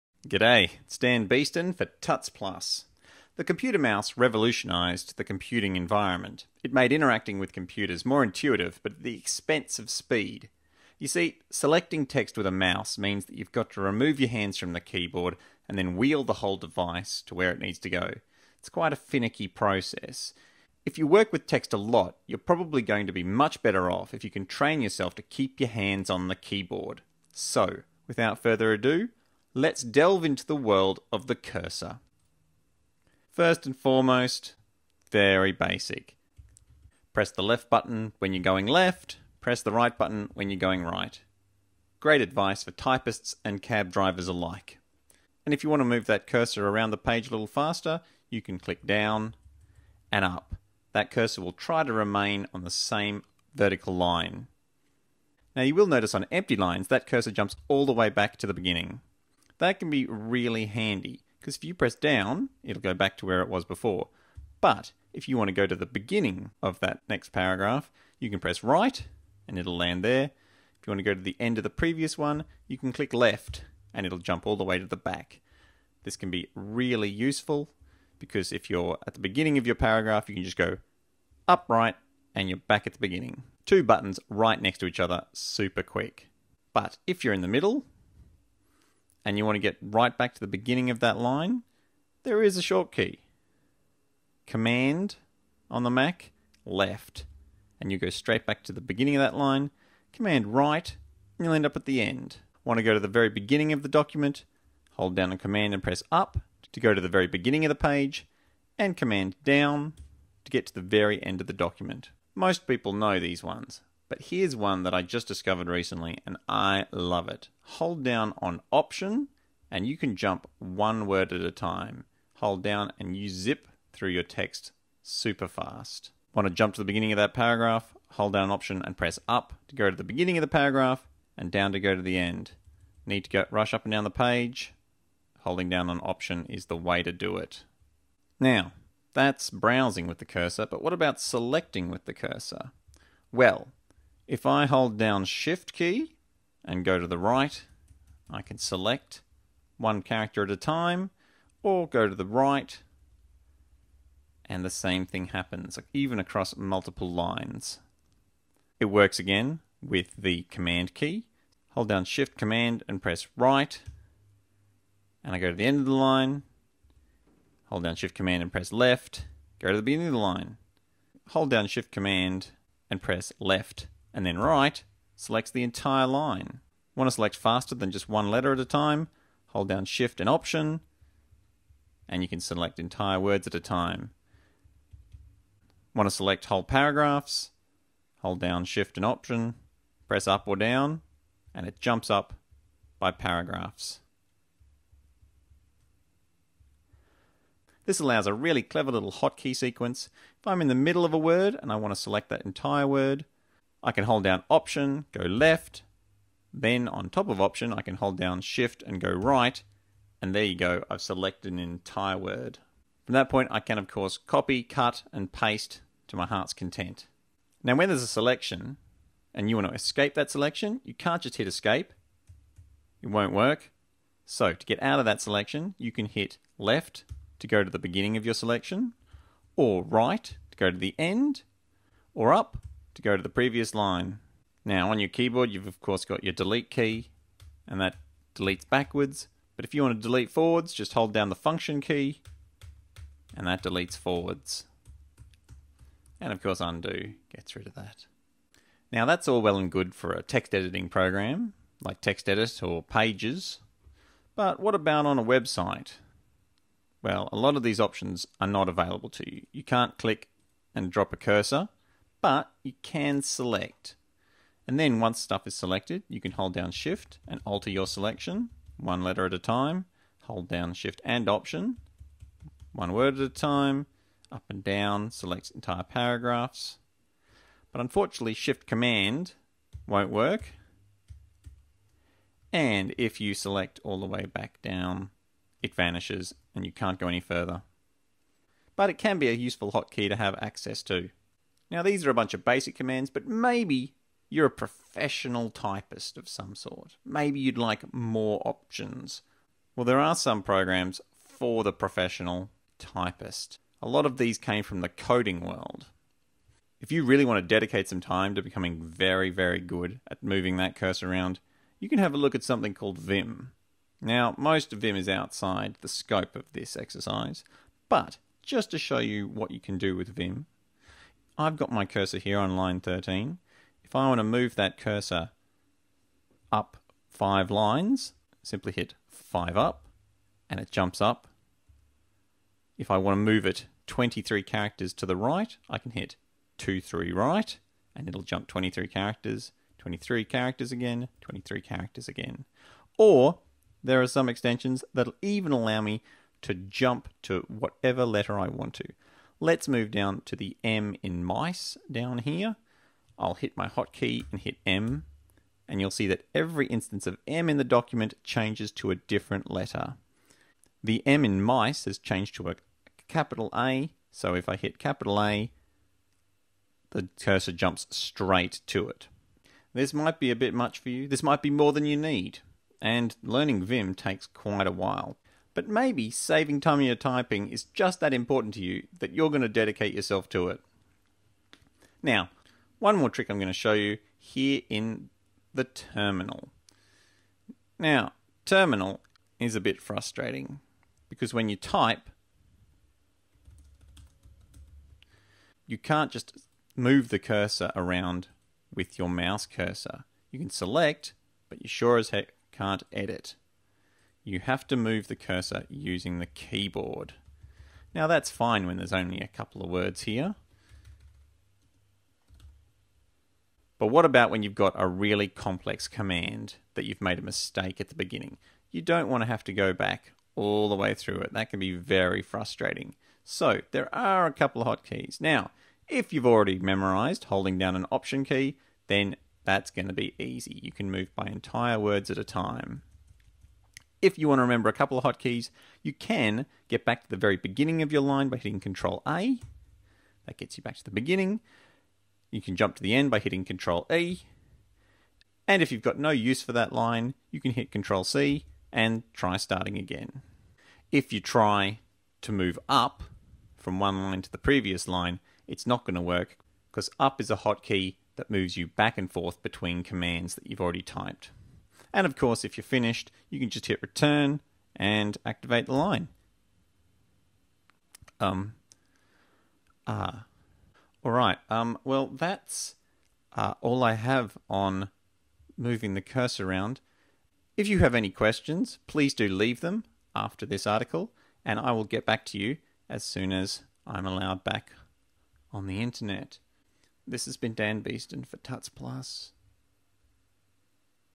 Move Your Keyboard Cursor Like a Ninja
It's faster to move your cursor with the keyboard than to drag the mouse around.

If you're editing your thesis, writing your great novel or typing commands into terminal, this screencast will tell you everything you need to know to move that little cursor from here to there in the shortest length of time.